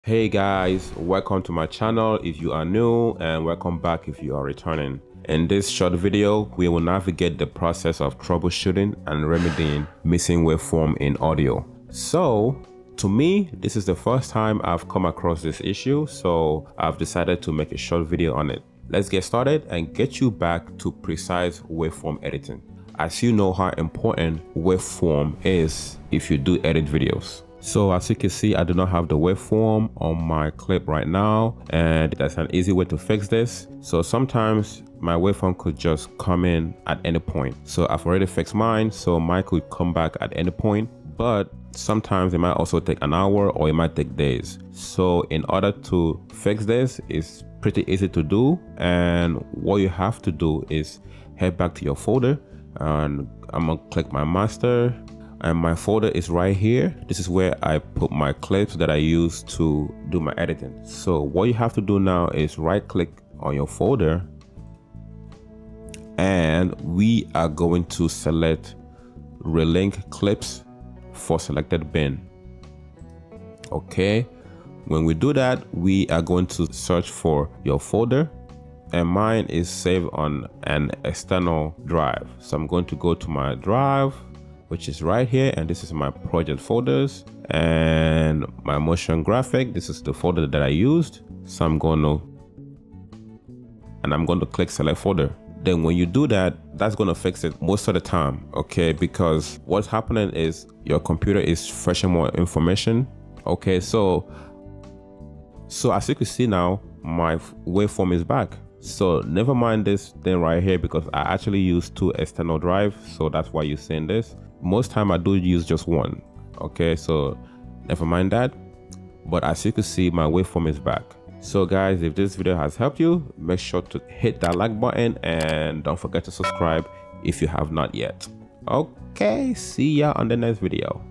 Troubleshooting: Restoring Missing Waveforms in DaVinci Resolve"
Encounter the challenge of missing waveforms in your DaVinci Resolve project? Fear not! In this comprehensive guide, we unveil effective techniques to address this issue and restore those crucial audio waveforms. From troubleshooting steps to pinpointing the cause, join us as we navigate the process of bringing your audio tracks back to life. Elevate your editing expertise and ensure a seamless audio-visual experience. Let's dive into the world of waveform recovery in DaVinci Resolve! 🎧🔍